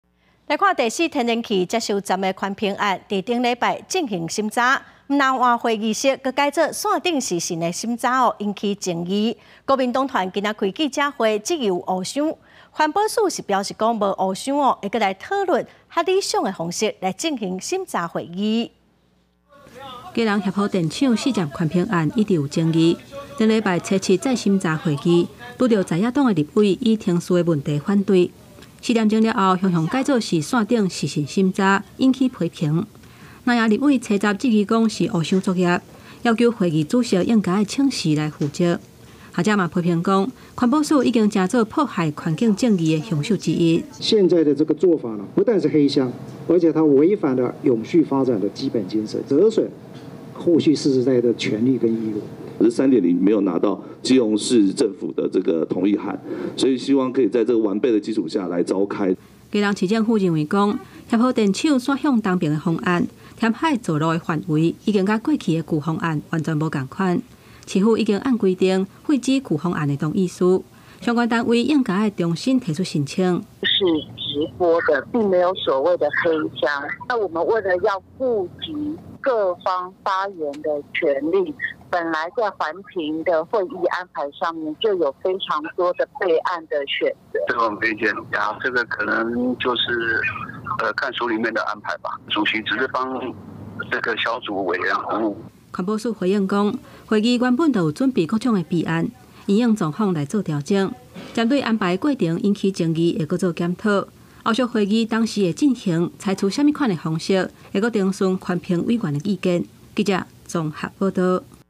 四接環評會臨時改視訊審查 藍營質疑使暗盤 | 公視台語台 | 20230710
第四天然氣接收站的環評案，在上禮拜進行審查，不只換會議室，閣改做線頂視訊審查，引發爭議，國民黨團今天開記者會，質疑有黑箱，環保署表示沒黑箱，會閣討論較適合的方式來進行審查會議。

四節新聞 ：　逐工（每天）09:00 / 12:00 / 15:00（拜一至拜五） / 19:00